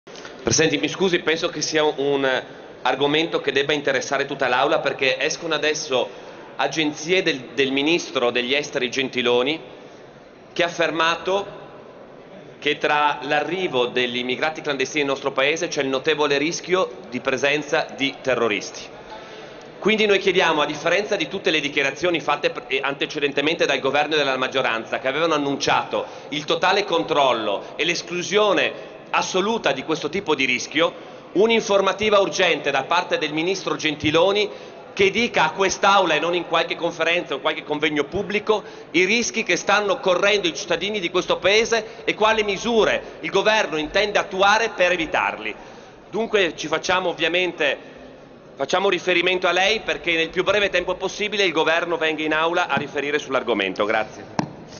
Fedriga: importiamo terroristi,Gentiloni riferisca in aula
ROMA, 22 GEN – “Visto che per voce dello stesso ministro degli Esteri è venuto a galla che il governo sta importando terroristi a spese degli italiani, La Lega Nord chiede un’informativa urgente affinché il ministro Paolo Gentiloni riferisca in Aula i rischi che stanno correndo i cittadini di questo Paese a causa dell’immigrazione indiscriminata e quali misure il governo intende attuare per evitarli. Purtroppo, quando la Lega Nord già molto tempo fa denunciò questo grave rischio, fu tacciata di razzismo da alcuni membri dello stesso governo”.

Lo scrive in una nota il capogruppo della Lega Nord alla Camera, Massimiliano Fedriga.